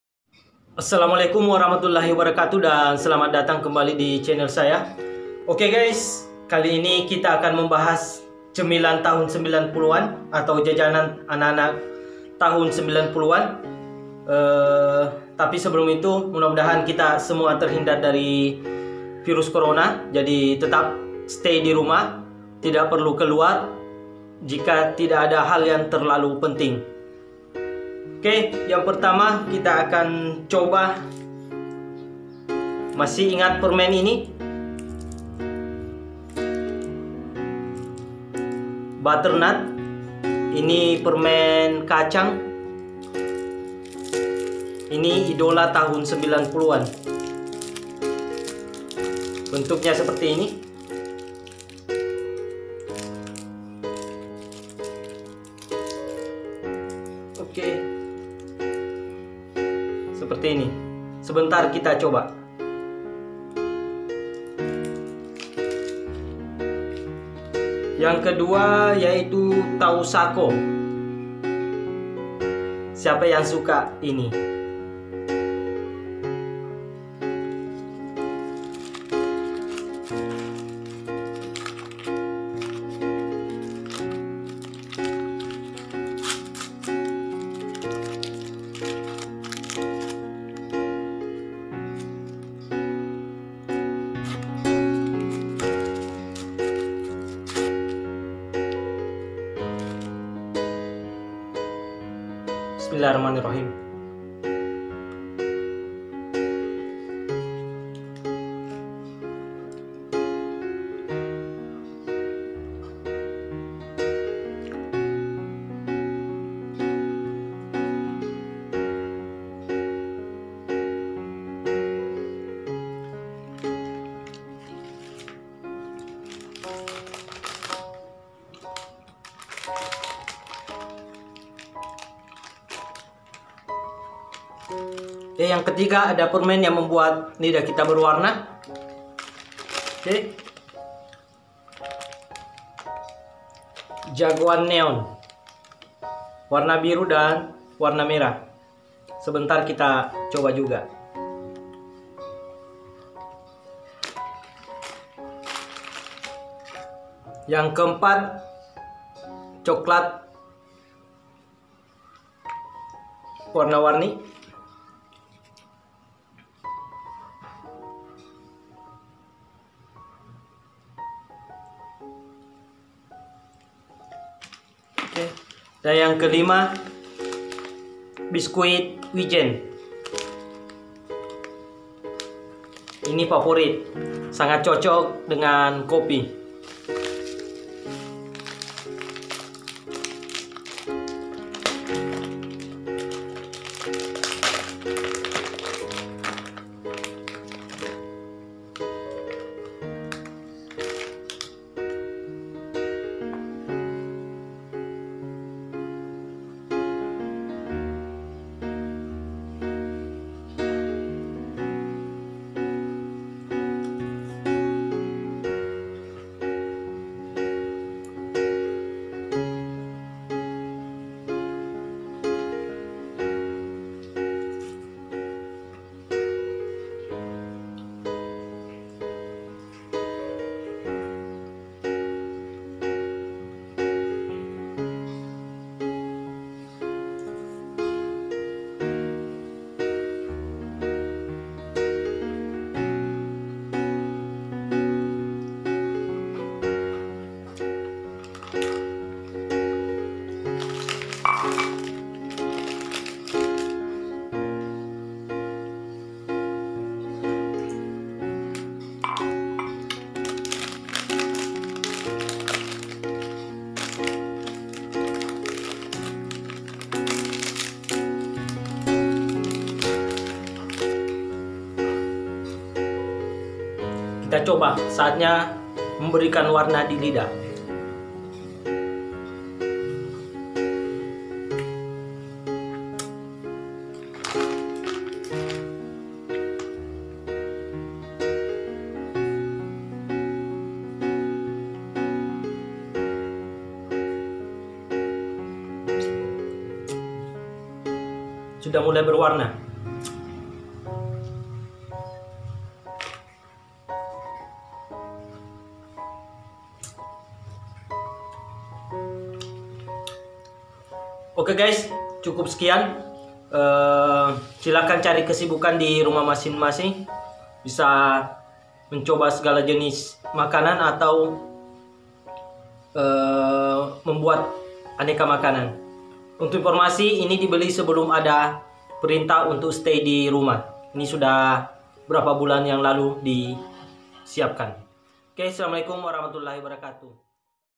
Jajanan 90an
kali ini saya menikmati jajanan tahun 90an, sambil mengingat masa2 kecil
selamat menonton, tetap stay dirumah semoga kita terhindar dari segala macam penyakit